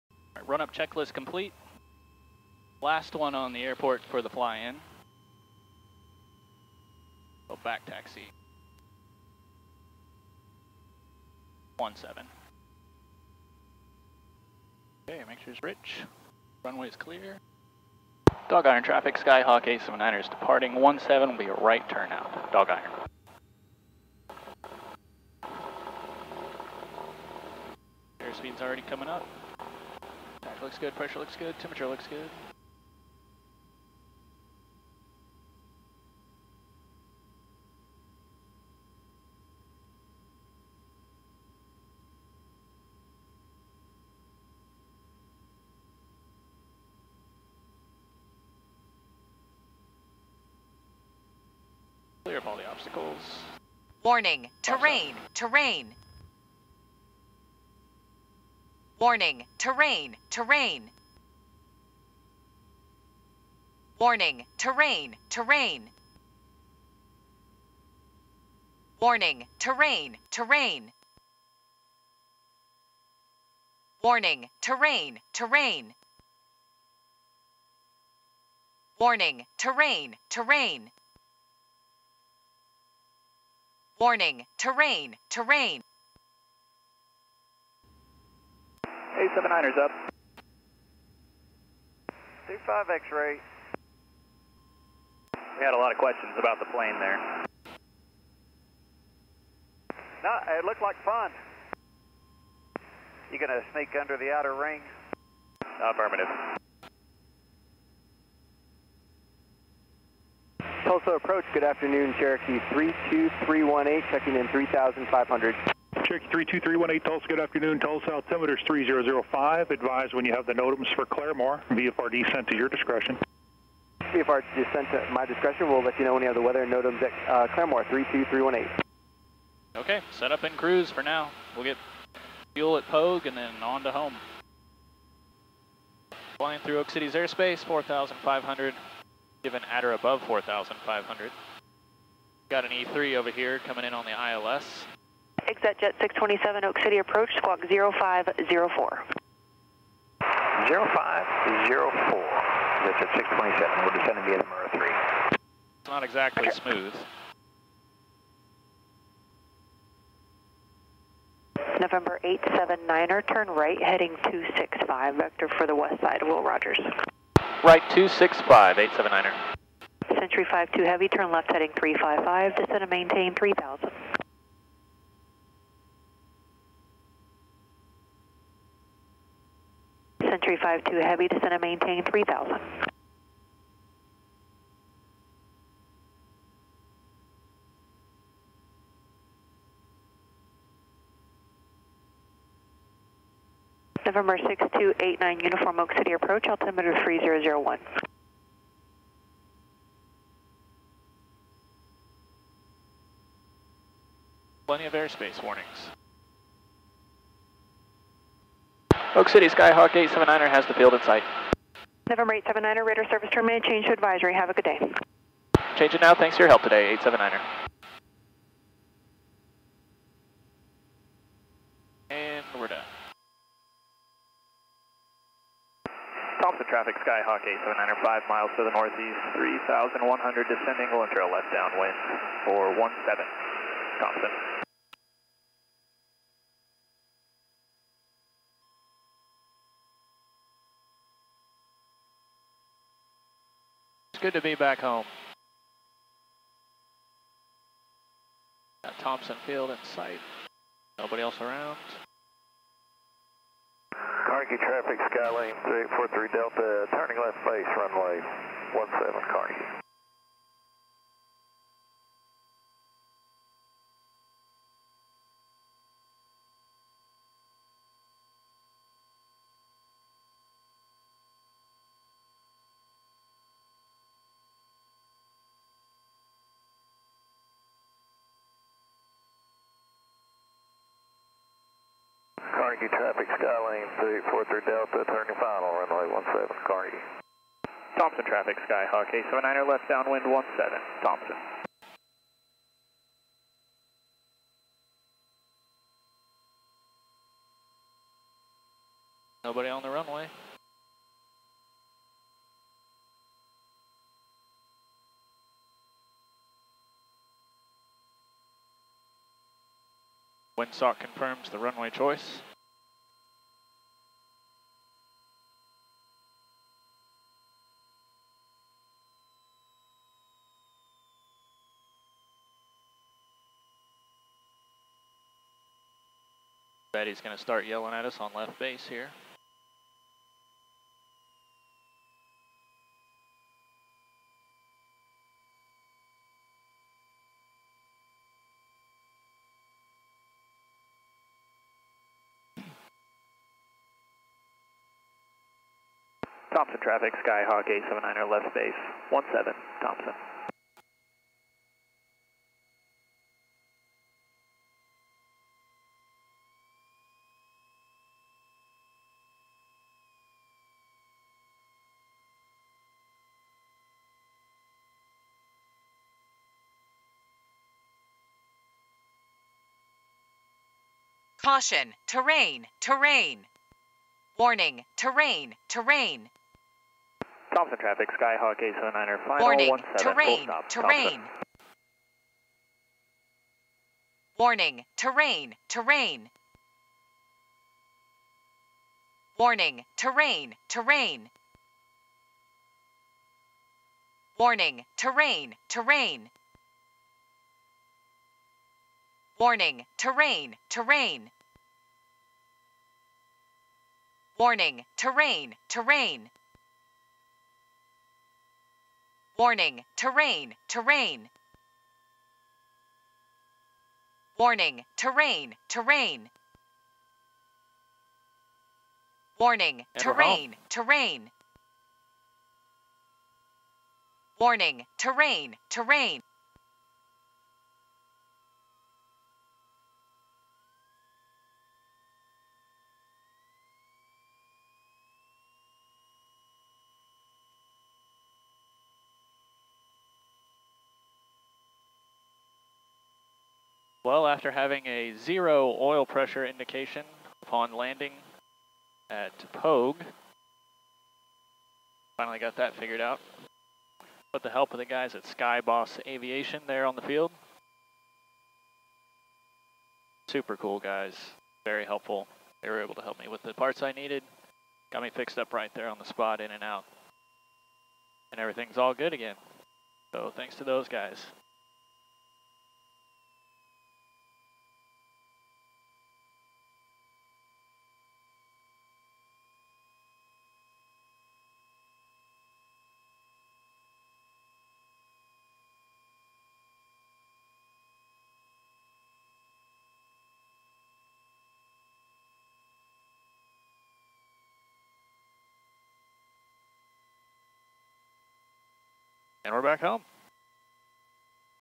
Departing Dog Iron Ranch -- Oologah Lake, OK
On this flight I depart the Will Rogers/Wiley Post Fly-In at Dog Iron Ranch. On the way back home I planned to stop at William R Pogue Municipal Airport (KOWP) for fuel. About five miles away from the airport I noticed the oil pressure had dropped below normal operating pressure. As I reduced throttle in the pattern to land I noticed the oil pressure had dropped to nearly 0psi.

After topping off with fuel I started the engine (still extremely low oil pressure) and taxied over to a nearby open hangar to see if they had some tools I could borrow. Luckily Nate was in the hangar doing some work on another plane and was nice enough to let me borrow a few tools to troubleshoot the issue. After a few minutes it was apparent what the issue was and Nate, with the help of Bill (owner of Skyboss Aviation), was able to help me track down the parts I needed.

Thanks to the help and generosity of Nate and Bill I was able to get back in the air within an hour instead of possibly days waiting on parts to be mailed. If you're ever in the Tulsa area and need any maintenance done check out Bill and Nate. Information below: